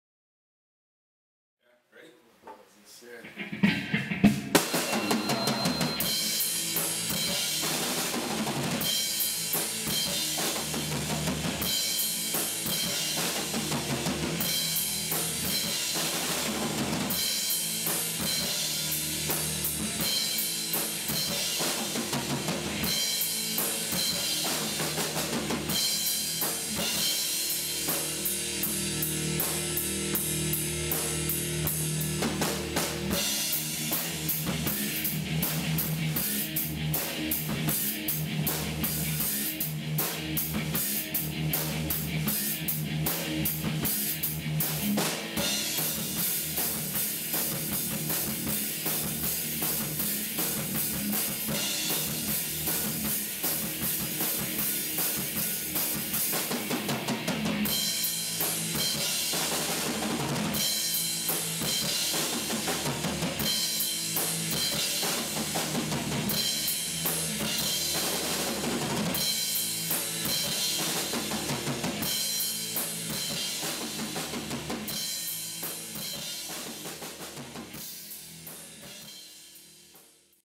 CATBOX Sessions 11/11/14
Working on some jams in the CATBOX!!

Red Beard - Drums
MorgallicA - Guitars